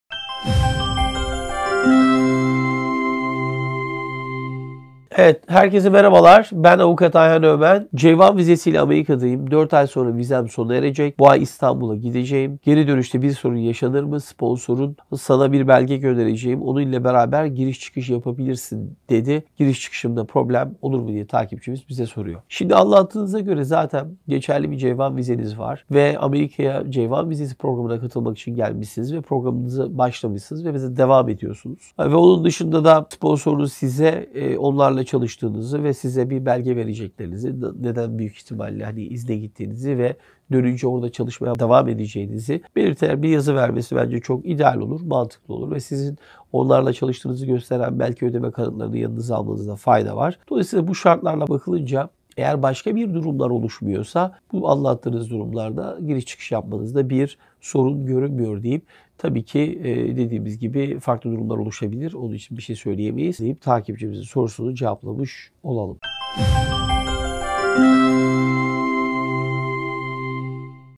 J1 VİZESİYLE AMERİKA’DAYKEN TÜRKİYE’YE GİDİŞTE DİKKAT EDİLMESİ GEREKENLER NELERDİR ?
Soru: J1 vizesiyle Amerika’dayım. 4 ay sonra vizem sona erecek. Bu ay İstanbul’a gideceğim, geri dönüşte bir sorun yaşanır mı ? Sponsorum sana bir belge göndereceğim onun ile beraber giriş çıkış yapabilirsin dedi. Giriş çıkışımda problem olur mu ?